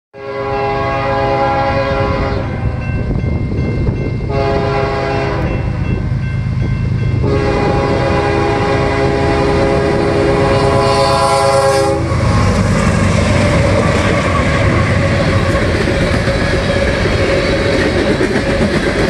CSXT 5214, 7738 & 3215 lead Q634 past Pomfret, NY
Thurs 10/19/17 1529 hours QD 44  Van Buren Road
LG V30 Video